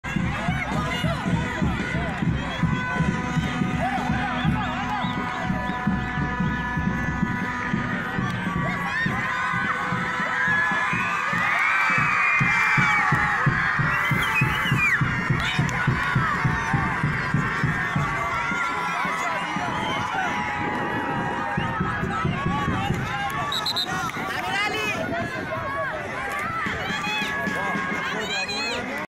Festival of Football Academies in Iran,soccer skills in youth player
Iranian Football Academy Festival, Precision Passes, Good Controls, Environmental Awareness, Good Placement